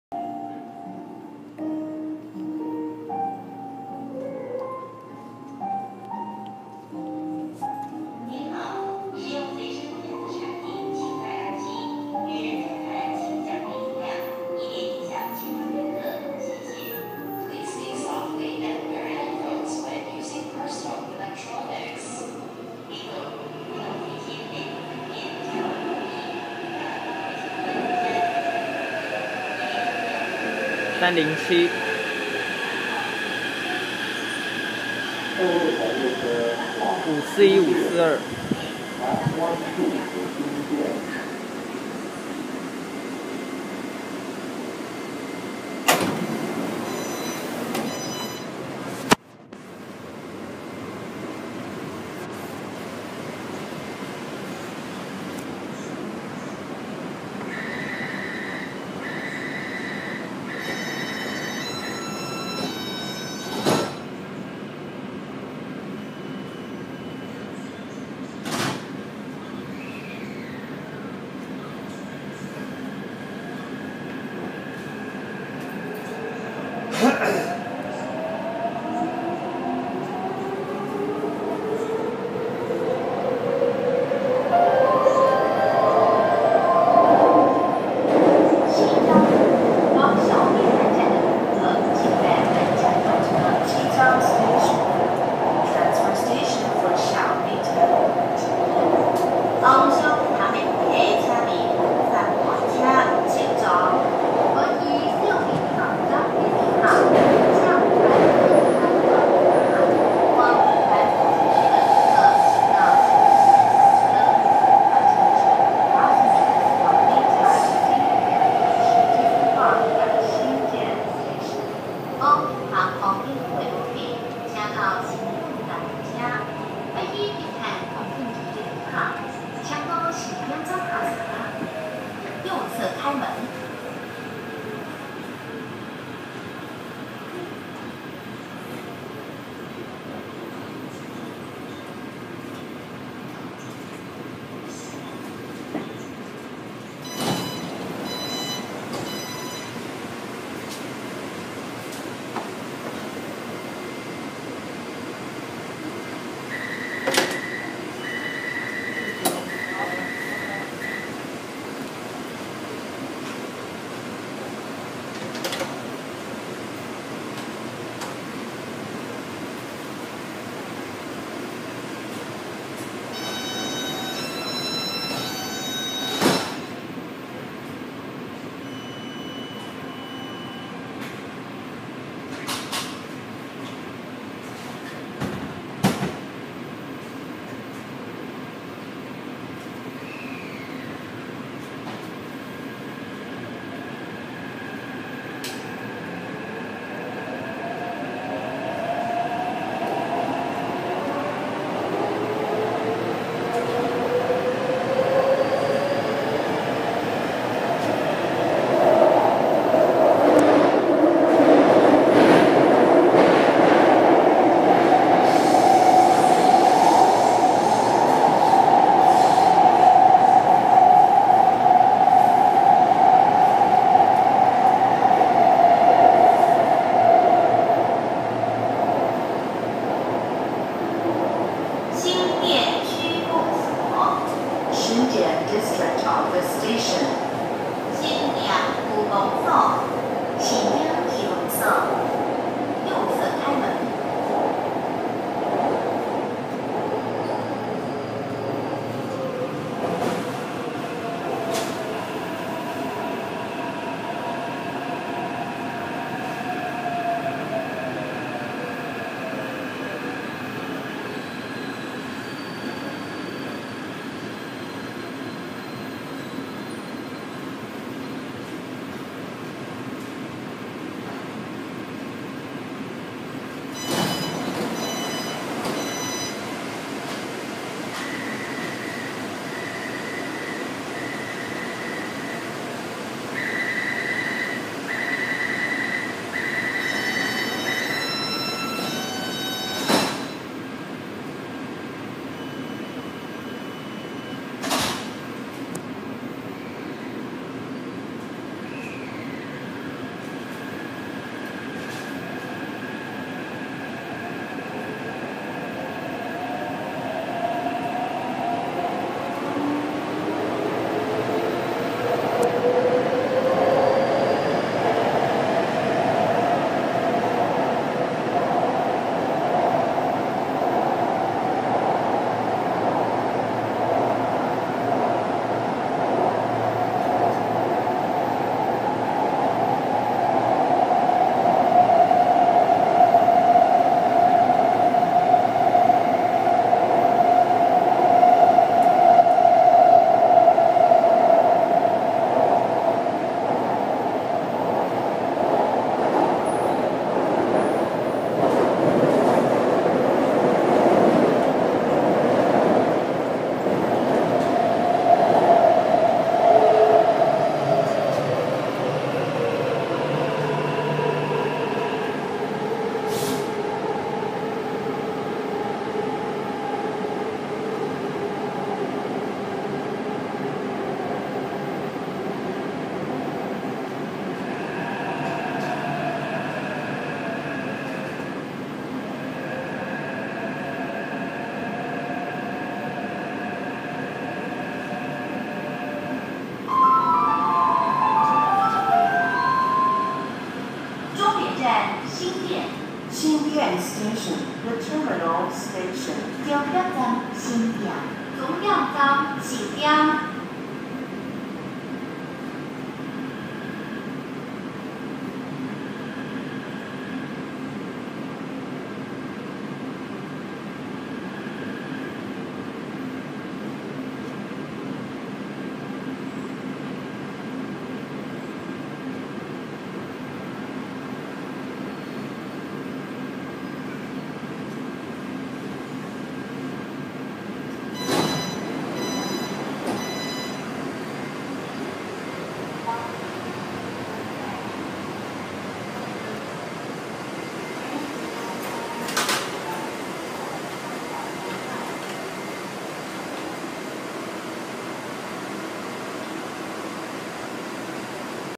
|交通迷天地| [台北捷運] C381型 541/542 往G01新店 G04/Y07大坪林~G01新店
拍攝日期: 2020年2月4日
拍攝路線: G松山新店線
車次: 307
車號: 541/542
車況: 待發